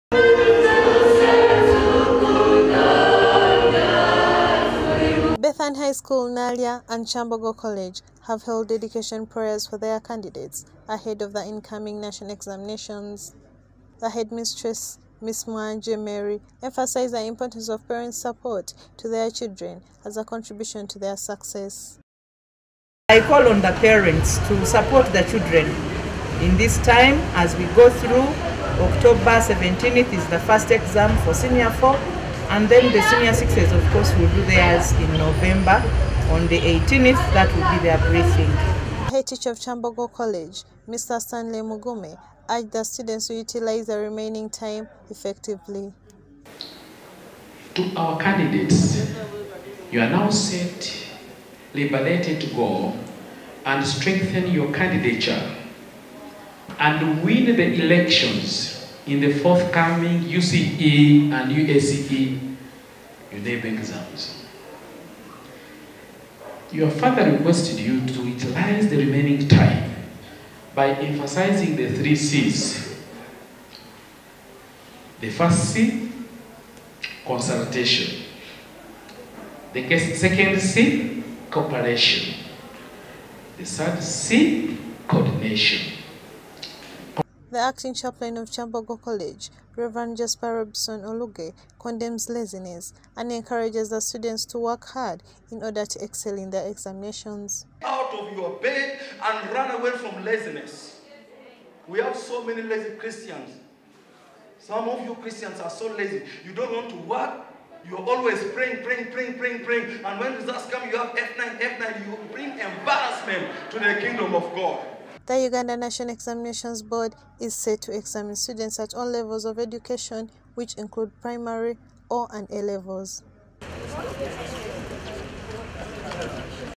Uganda National Examinations Board is set to examine students at all levels
As Uganda National Examinations draw close, students have been advised to work hard and cooperate with one another in order pass their examinations.
This was during the dedication services that were held in both                                                                  Bethany High school, and Kyambogo College